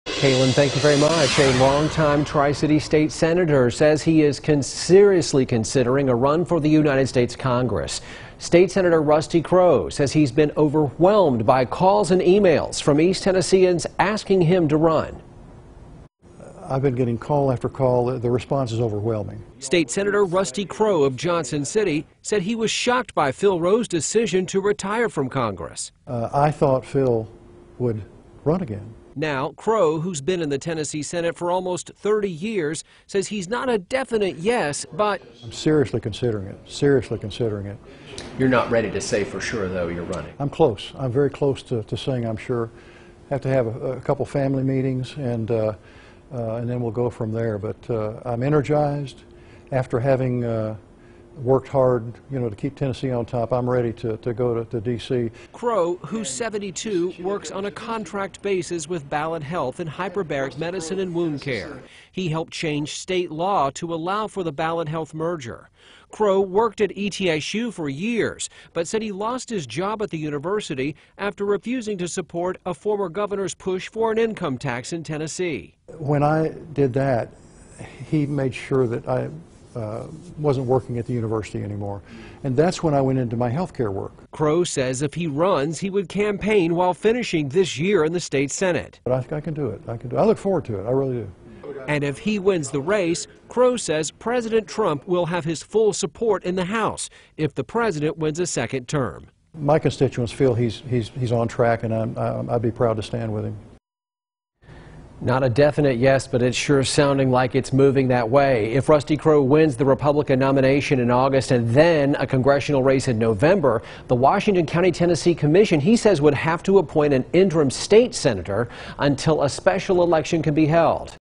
State Sen. Rusty Crowe 'seriously considering' running for Congress
Tennessee state Sen. Rusty Crowe (R-Johnson City) says he's seriously considering a run for the United States Congress.